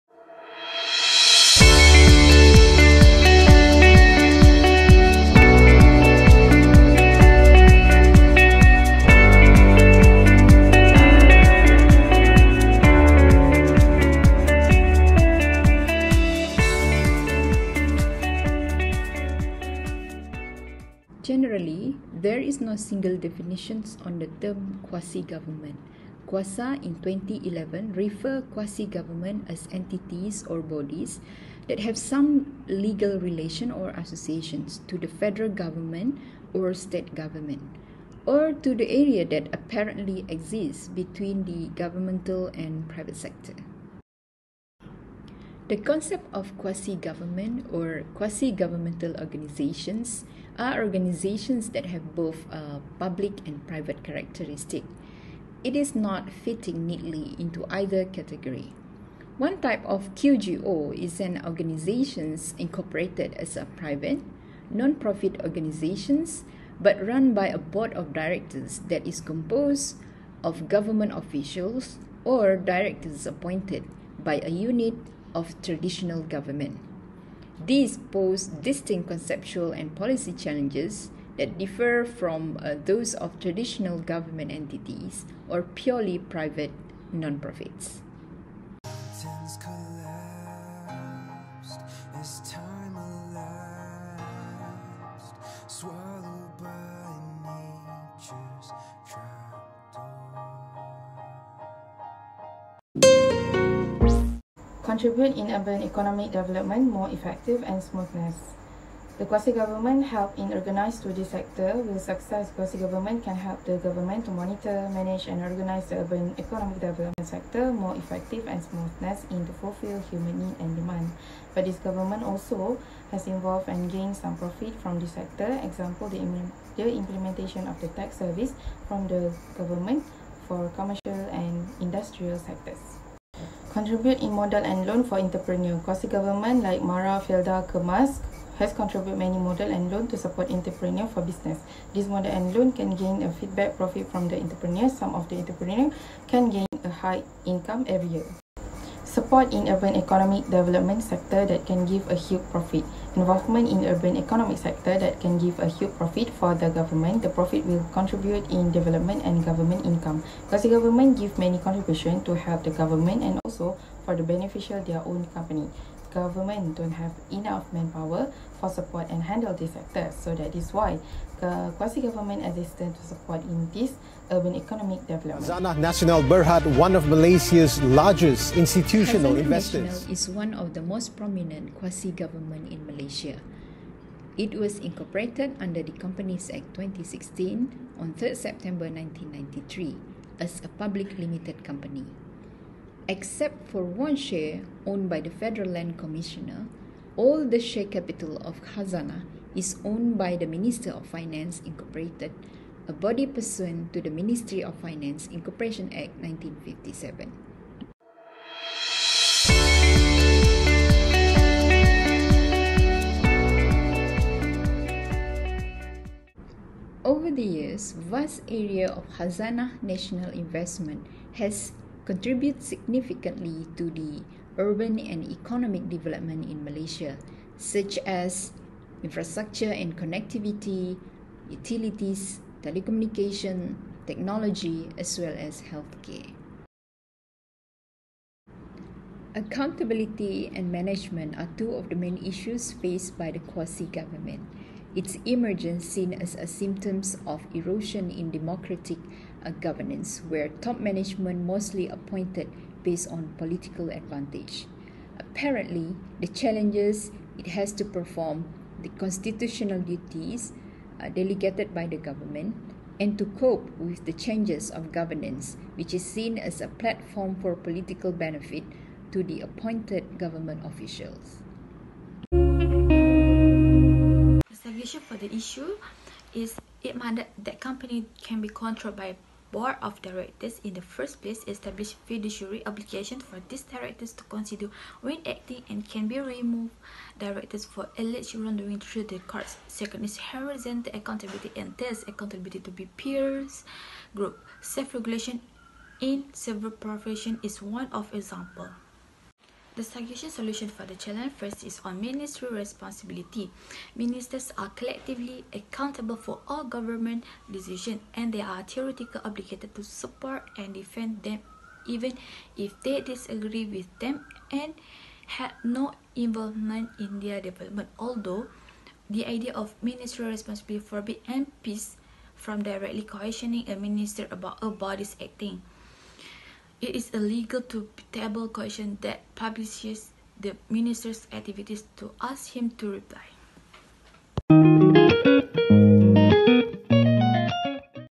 QUASI GOVERNMENT
Master of Science in Urban Development and Management
Faculty of Architecture, Planning and Surveying
Universiti Teknologi Mara, Shah Alam.

Subject Code : UDM722
Subject name : Urban Governance and Finance

Prepared by :
1) Suljirina Lucas
2) Nurul Nathasa Mat Mansor
3) Nur Hannan Binti Abd Basir

Prepared for :
Dr Mohd Azren Hassan